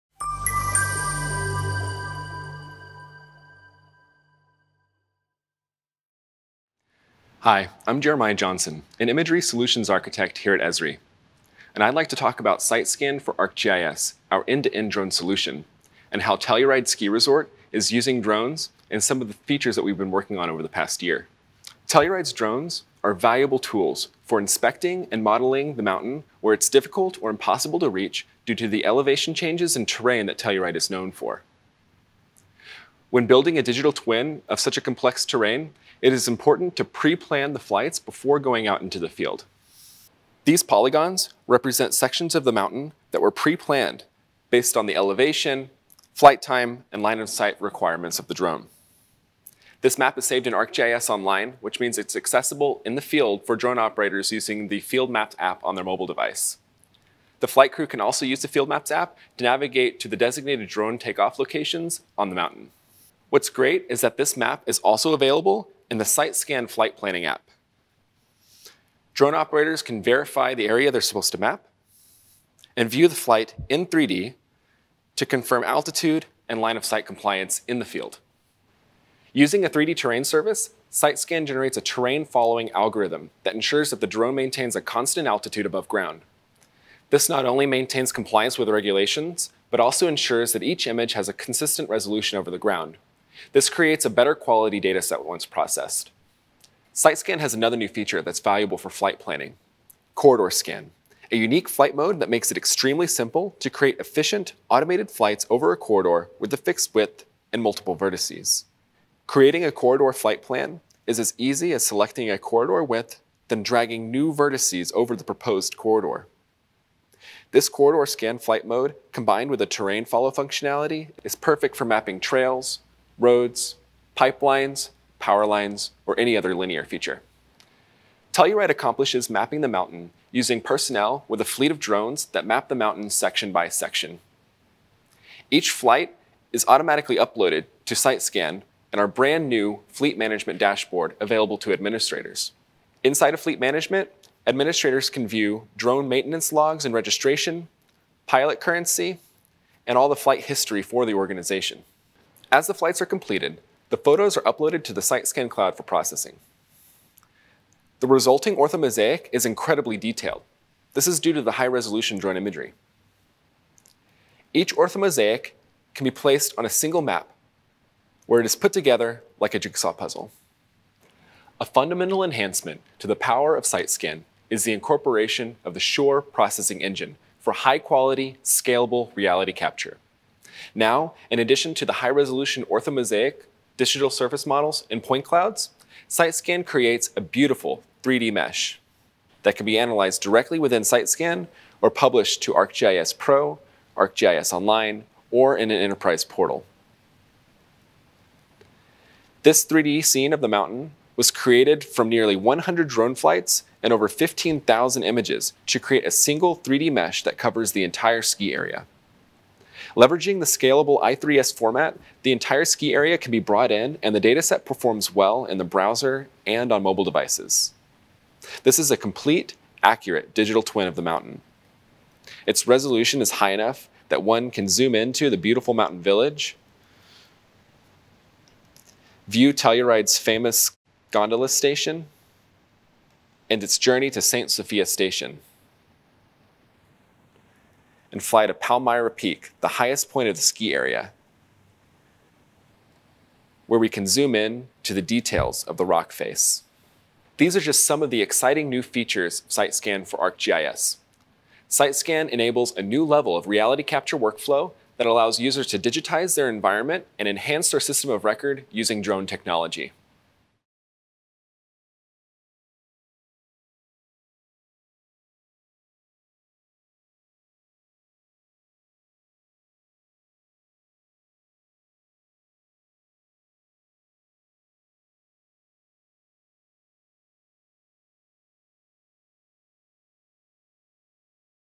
Reality Capture & Site Scan for ArcGIS
Take flight with new capabilities from Site Scan for ArcGIS.  Esri's Jeremiah Johnson shows you how.